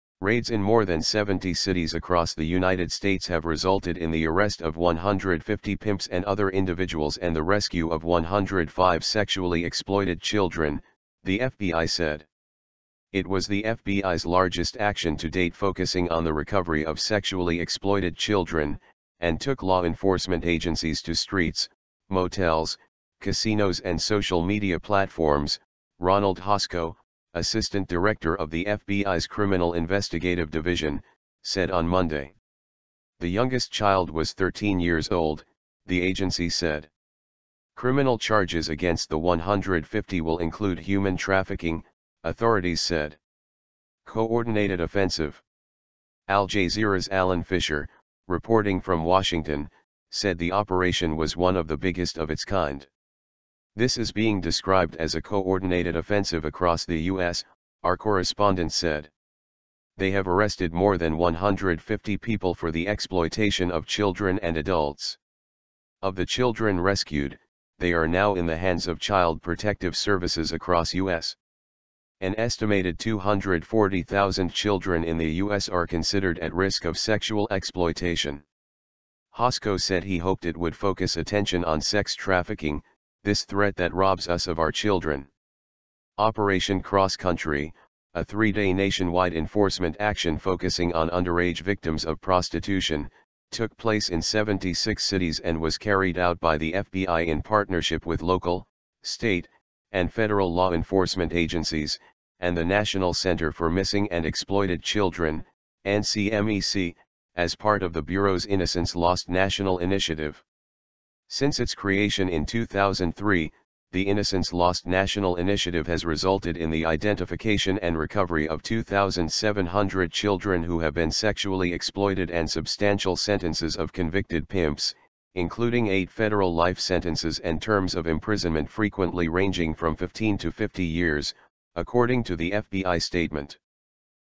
FBI Arrests 150 Pimps In United States
Raids in more than 70 cities across the United States have resulted in the arrest of 150 "pimps" and other individuals and the rescue of 105 sexually exploited children, the FBI said.

"It was the FBI's largest action to date focusing on the recovery of sexually exploited children, and took law enforcement agencies to streets, motels, casinos and social media platforms", Ronald Hosko, assistant director of the FBI's criminal investigative division, said on Monday.

The youngest child was 13 years old, the agency said.

Criminal charges against the 150 will include human trafficking, authorities said.

'Co-ordinated offensive'

Al Jazeera's Alan Fisher, reporting from Washington, said the operation was one of the biggest of its kind.

"This is being described as a co-ordinated offensive across the US," our correspondent said.

"They have arrested more than 150 people for the exploitation of children and adults. Of the children rescued, they are now in the hands of child protective services across US."

An estimated 240,000 children in the US are considered at risk of sexual exploitation.

Hosko said he hoped it would focus attention on sex trafficking, "this threat that robs us of our children".

Operation Cross Country - a three-day nationwide enforcement action focusing on underage victims of prostitution - took place in 76 cities and was carried out by the FBI in partnership with local, state, and federal law enforcement agencies, and the National Center for Missing and Exploited Children (NCMEC) as part of the Bureau's Innocence Lost National Initiative.

Since its creation in 2003, the Innocence Lost National Initiative has resulted in the identification and recovery of 2,700 children who have been sexually exploited-and substantial sentences of convicted pimps, including eight federal life sentences and terms of imprisonment frequently ranging from 15-50 years, according to the FBI statement.